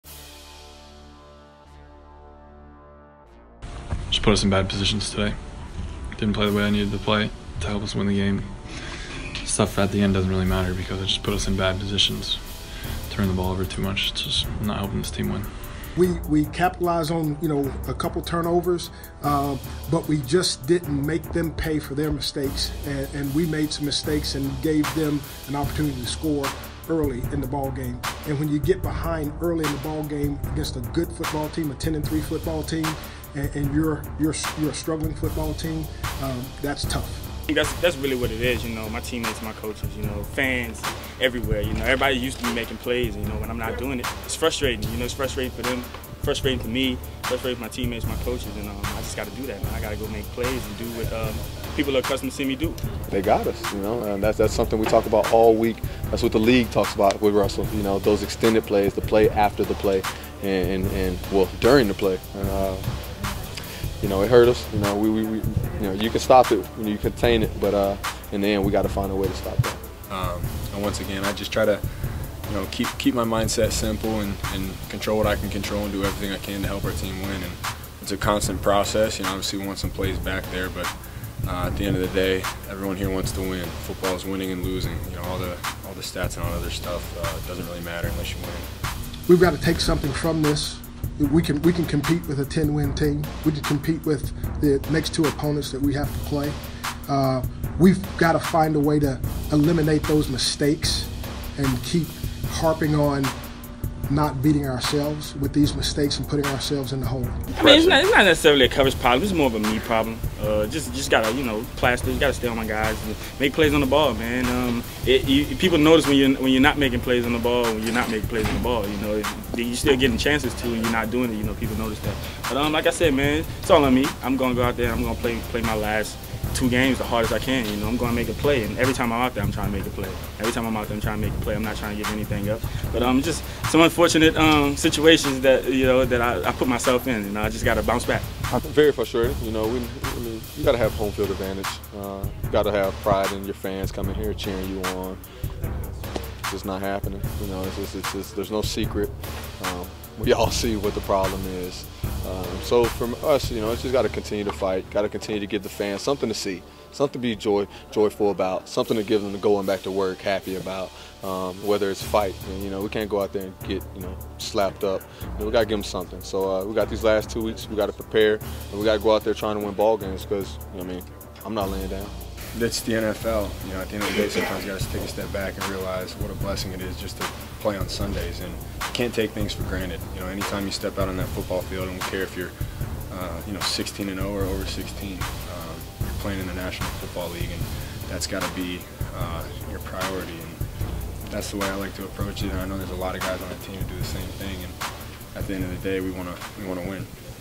Panthers Fall To Seahawks, Lose Sixth Straight
Donte Jackson, Kyle Allen, Tre Boston, Christian McCaffrey and Perry Fewell spoke about the team's 30-24 loss to the Seahawks.